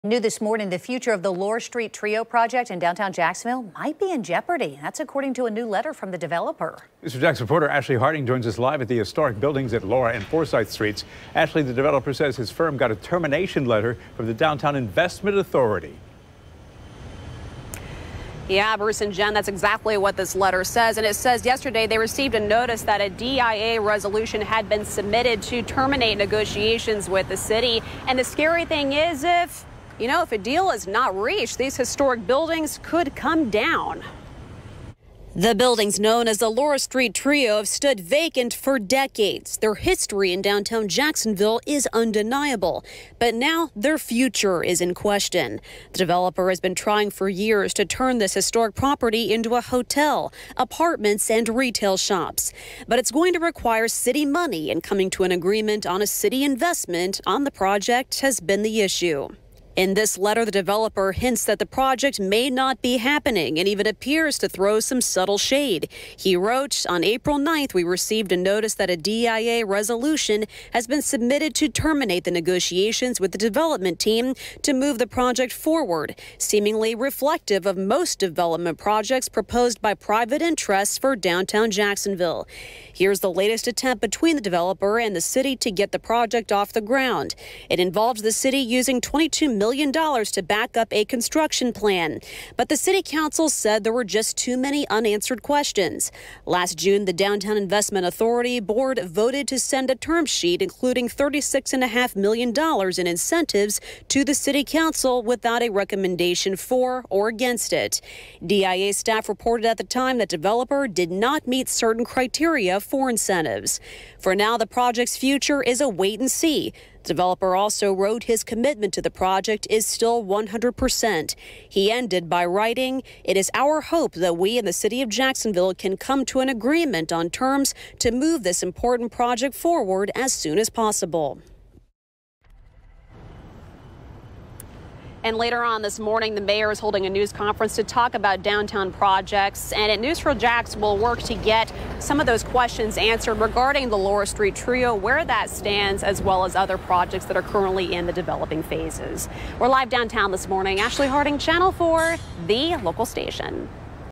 Developer says DIA wants to end negotiations for Laura Street Trio
The future of the Laura Street Trio project in downtown Jacksonville might be in jeopardy after the developer said his firm got a termination letter from the Downtown Investment Authority related to the historic buildings at Laura and Forsyth streets.